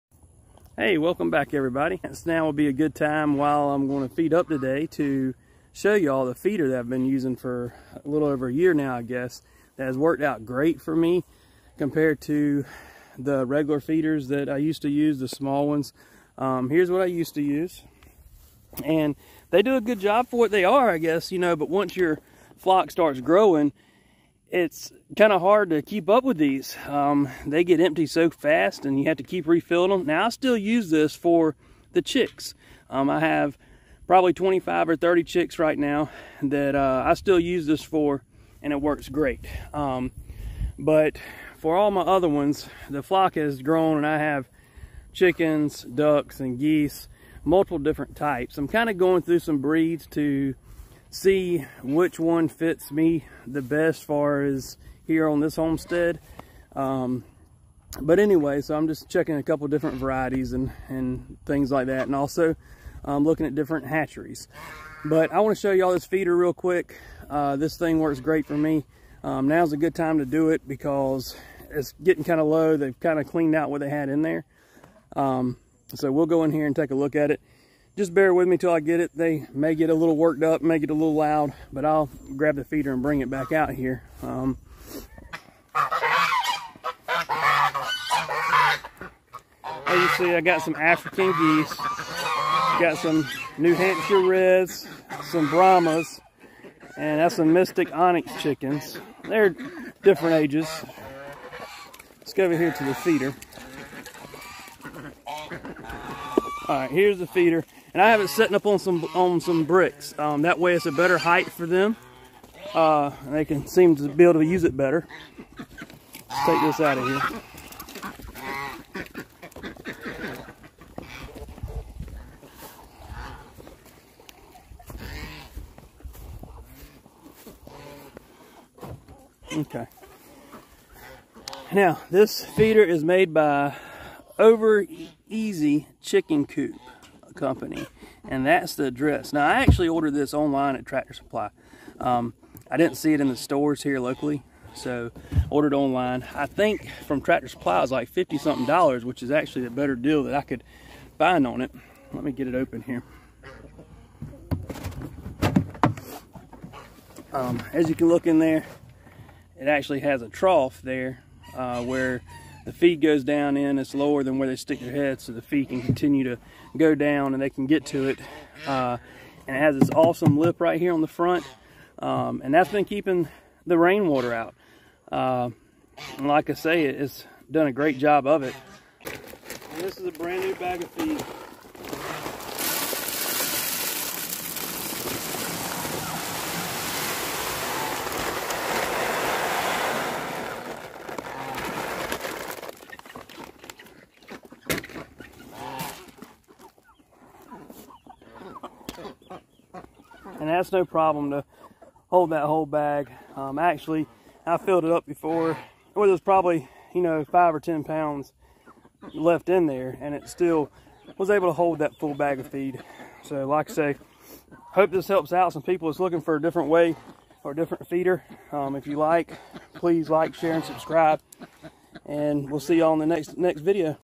Favorite feeder for Chickens, Ducks and Geese!
This feeder has saved me so much time with refills compared to using the regular feeder I was use to. It's also allowed me to keep my feed dry in the weather. It has definitely been the best one I've used.